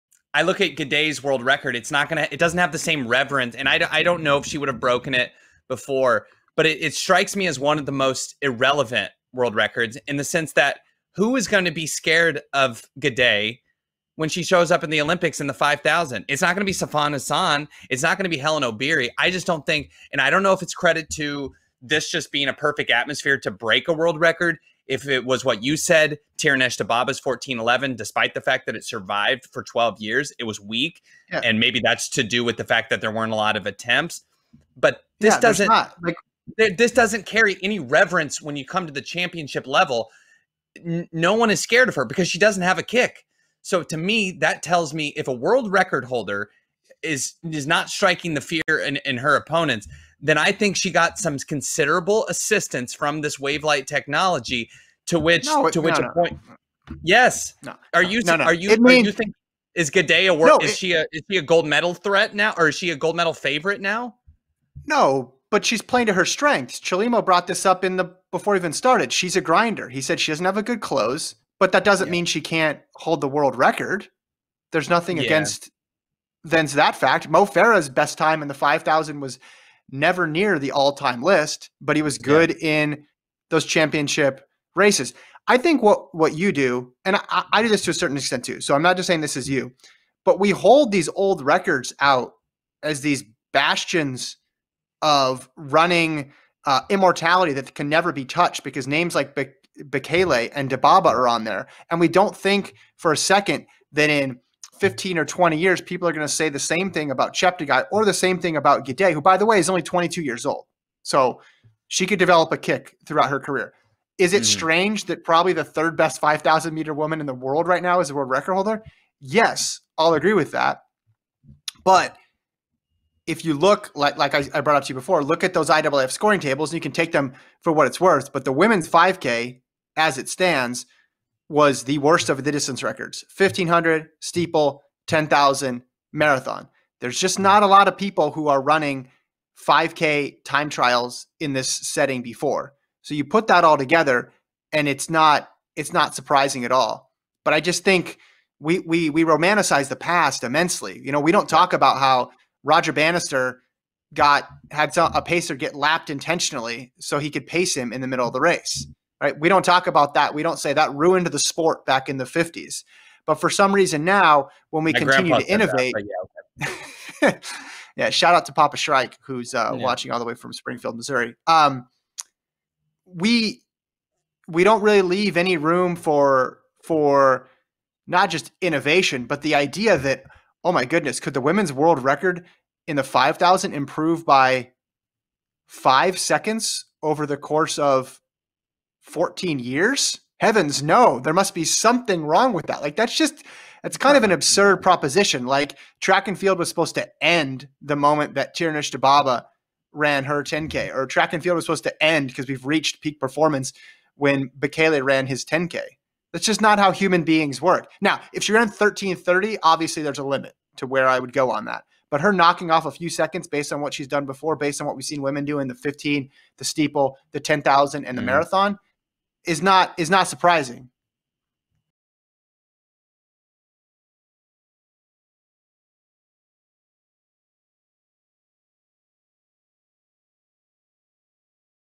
Why Letesenbet Gidey's World Record Is Different Than Others
Kevin and Lincoln dissect Letesenbet Gidey's 14:06 5,000m world record and how it might change her perception going forward.